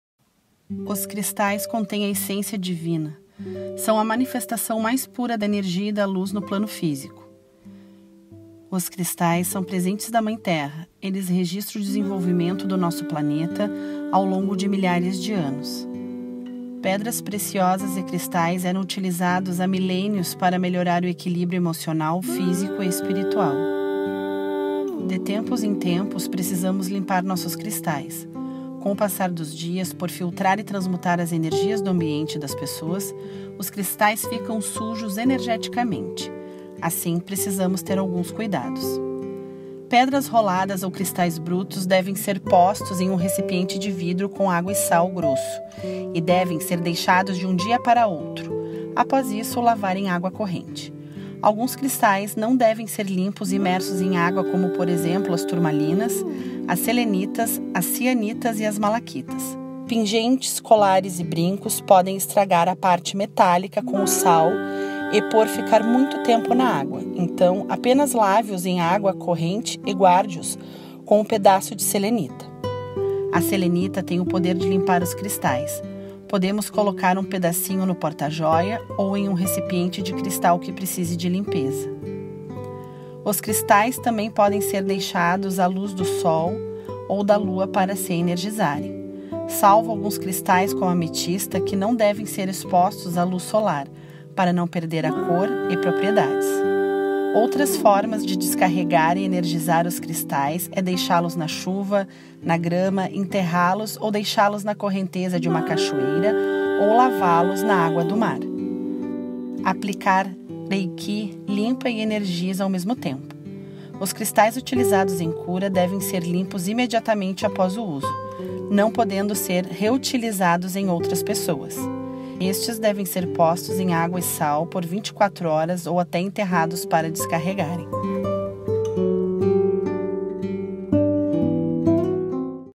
Como Limpar os Cristais
COMO LIMPAR E ENERGIZAR SEUS CRISTAIS

De tempos em tempos precisamos limpar nossos cristais. Com o passar dos dias, por filtrar e transmutar as energias do ambiente e das pessoas, os cristais ficam "sujos energeticamente". Assim, precisamos ter alguns cuidados.
Pedras roladas ou cristais brutos podem ser postos em um recipiente de vidro, com água e sal grosso e devem ser deixados de um dia para o outro, após isso, lavar em água corrente. Pingentes, colares e brincos podem estragar a parte metálica com o sal e por ficar muito tempo na água, então, apenas lave-os em água corrente.
Os cristais também podem ser deixados a Luz do Sol ou da Lua para se energizarem. Salvo alguns cristais, como a Ametista, que não deve ser exposta a luz solar para não perder a cor e propriedades.
Outras boas formas de energizar os seus cristais é deixá-los na chuva, grama, enterrá-los ou deixá-los na correnteza de uma cachoeira ou lavá-los na água do mar. Uma boa maneira para limpar e energizar os cristais é aplicando Reiki.